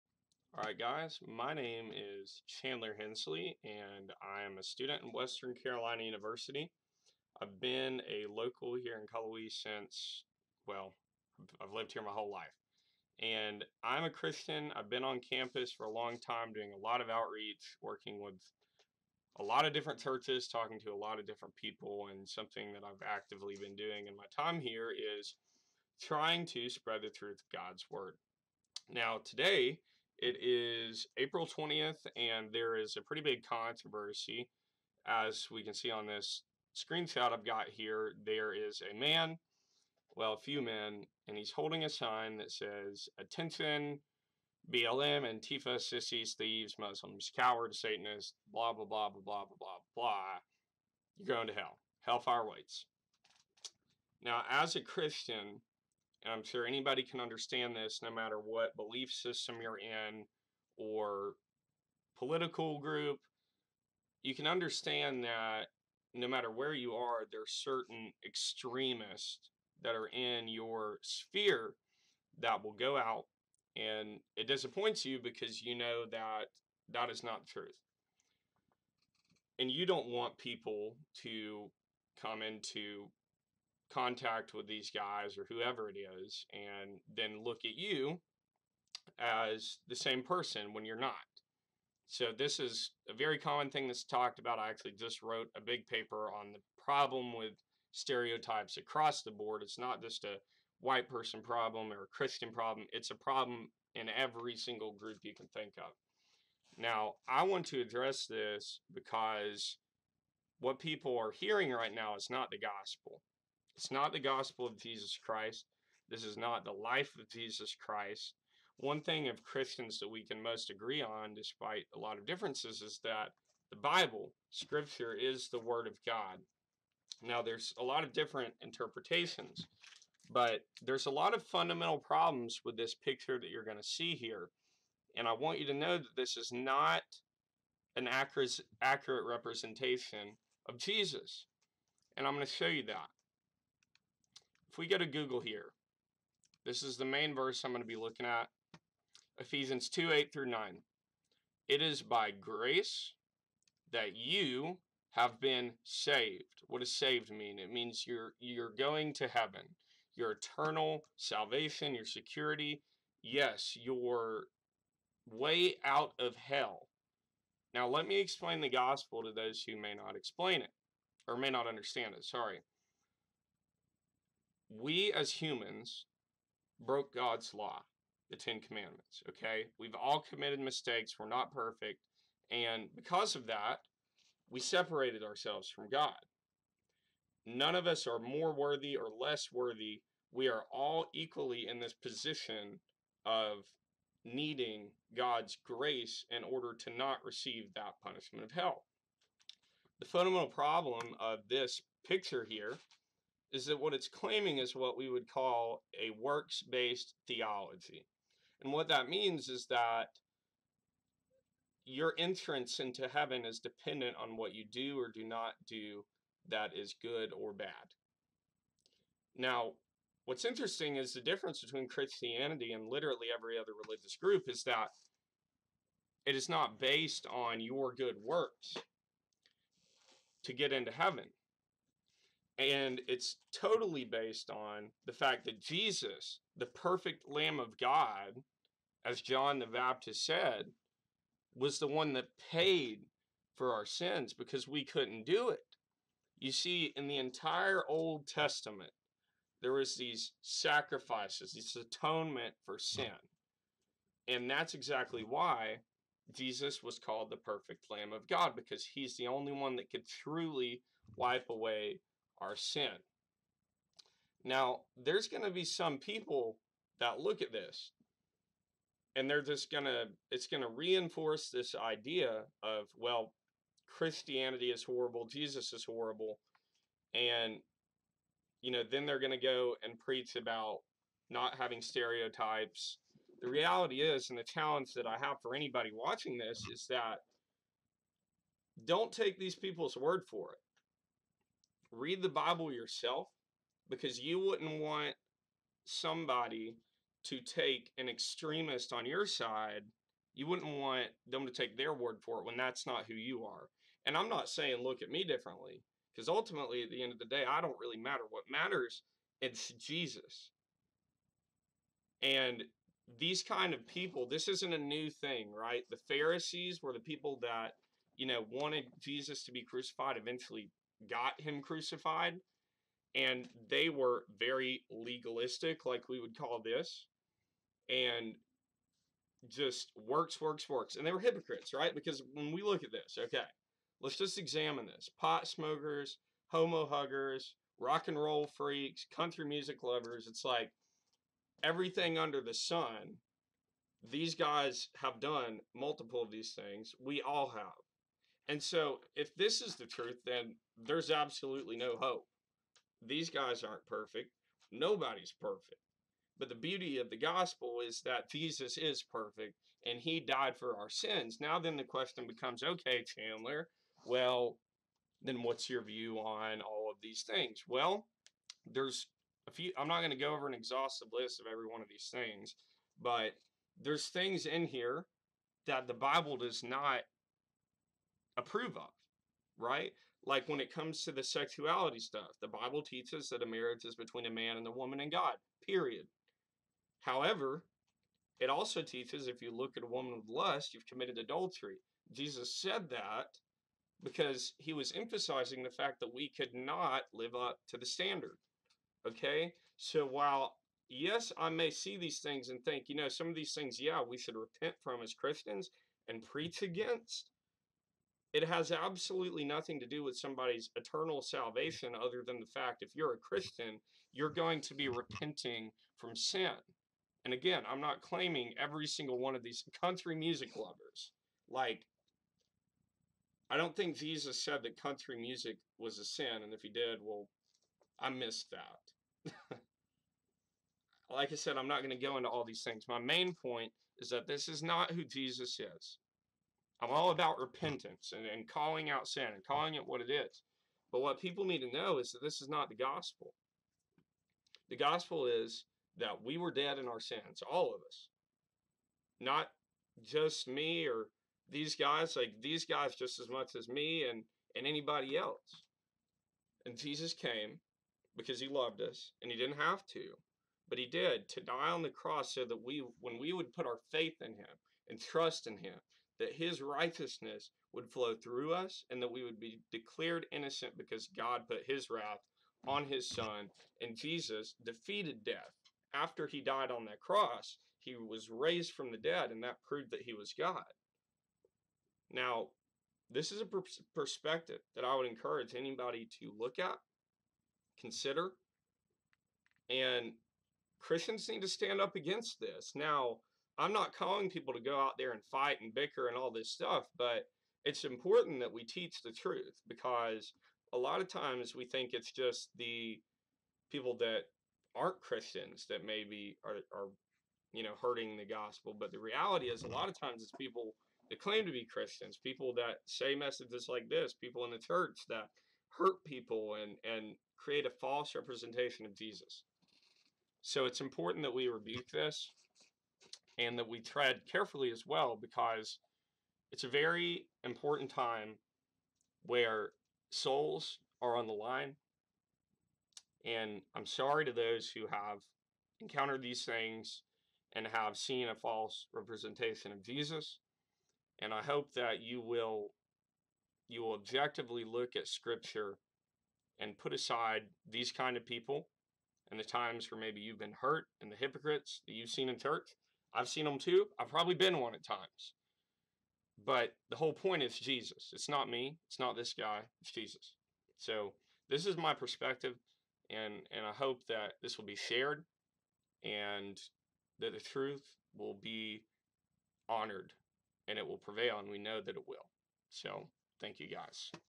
Street Preacher Controversy - Part 1 (My response)
Street Preachers are causing tension between students of all backgrounds at Western Carolina University.